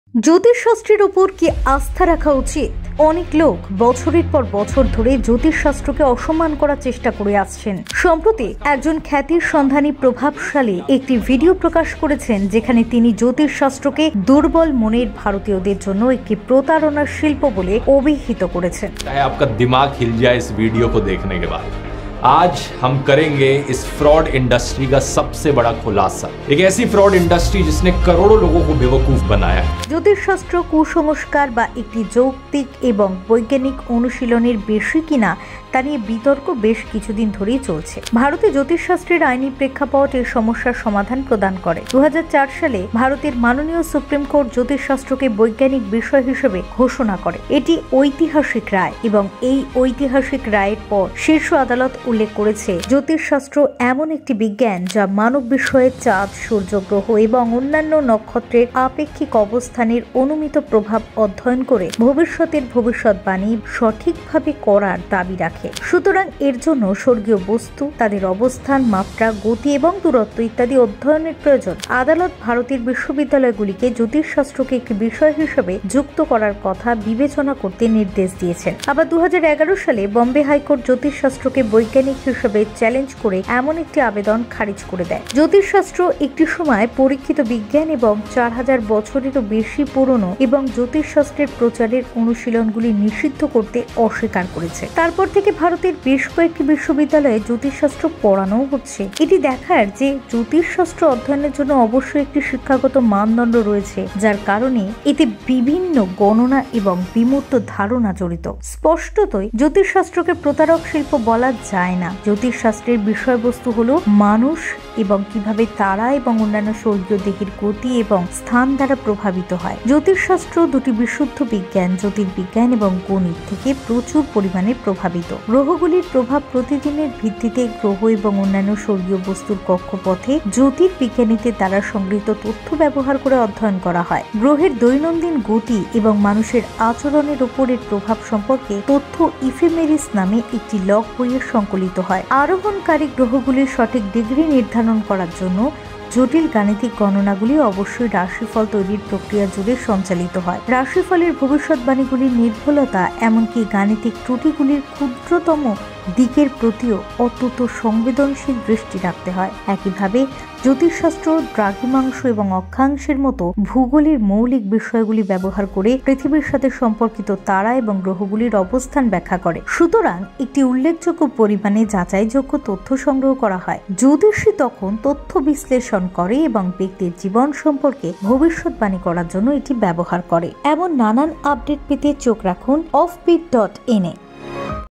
astrology : জ্যোতিষশাস্ত্র- এর ওপর কী আস্থা রাখা উচিত? off-bit.in
জ্যোতিষশাস্ত্র সর্বত্র একটি খুব জনপ্রিয় অনুশীলন, বিশেষ করে ভারতে। কিন্তু এর চেয়েও বেশি জনপ্রিয় হল এর সত্যতা নিয়ে বিতর্ক। জ্যোতিষশাস্ত্র কি আসল নাকি নকল? কিছু মানুষ কিভাবে সঠিক ভবিষ্যদ্বাণী করতে সক্ষম হয়? off-bit.in